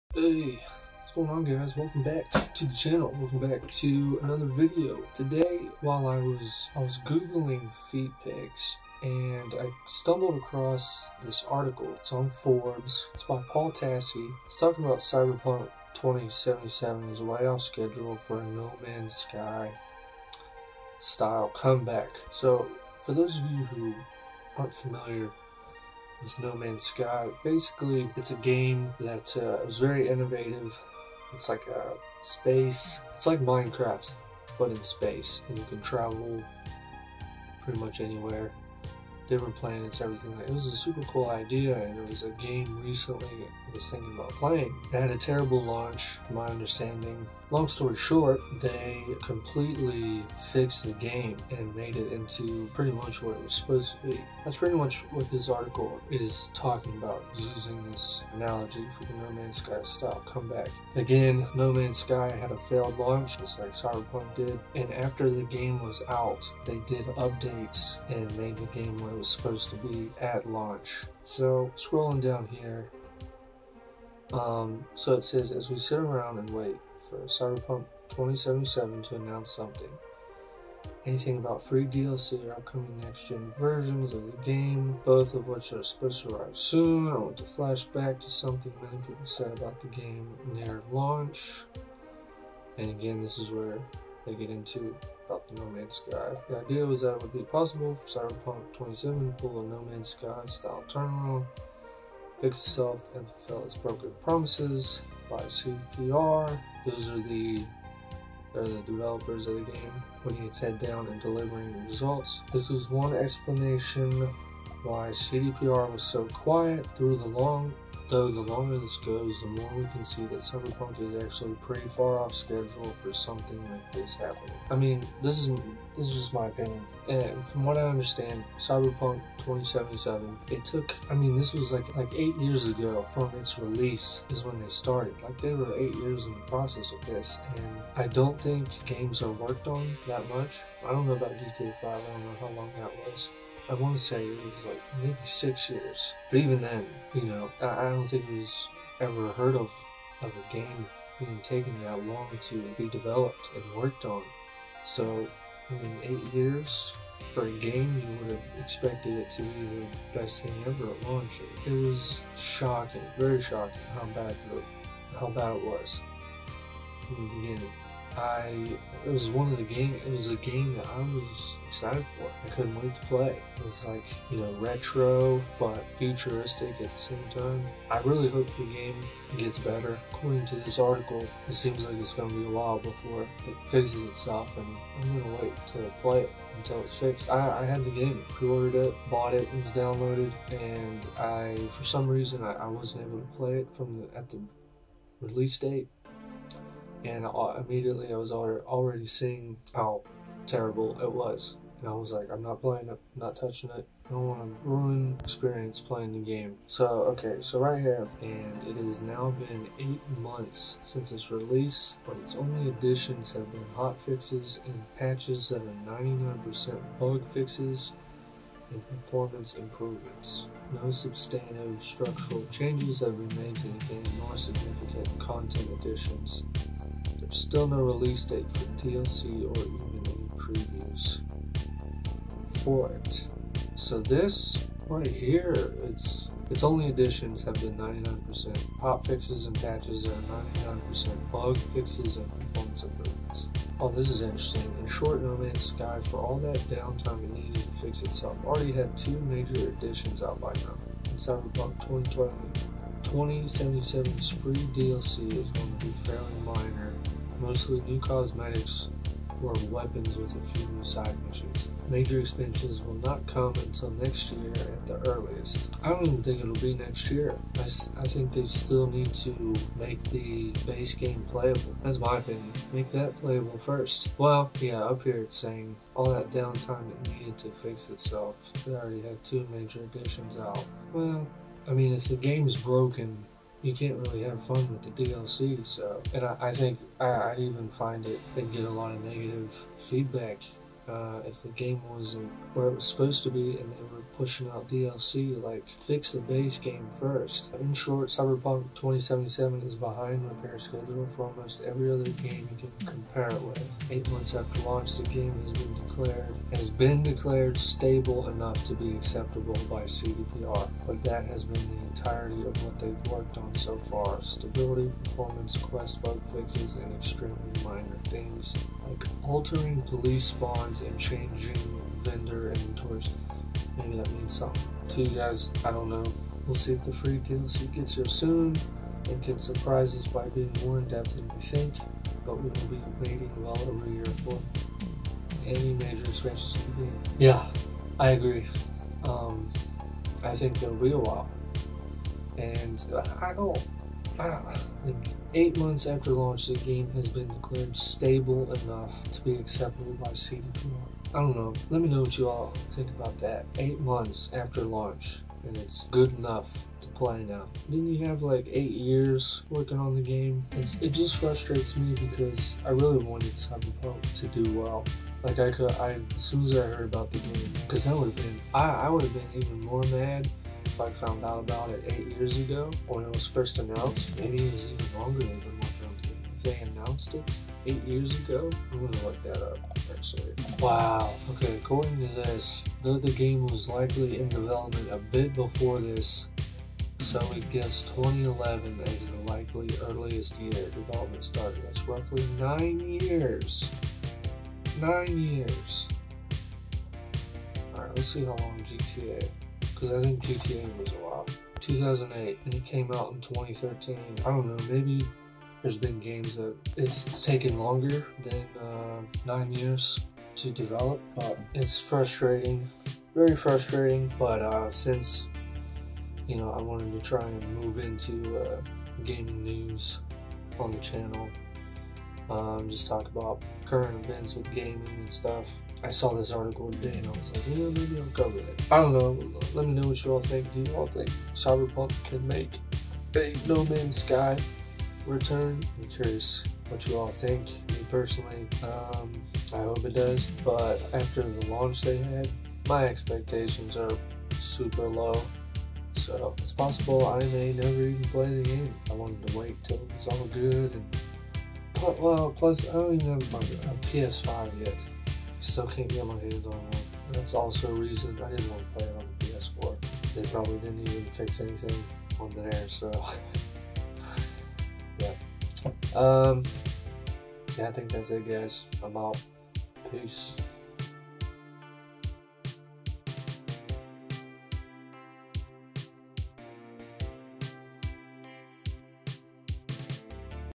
Can CYBERPUNK 2077 Make a COMEBACK Like NO MAN'S SKY??
Today guys I review an article on Forbes talking about if Cyberpunk 2077 can make a comeback like No Man's Sky once did.  What do you all think? Can they?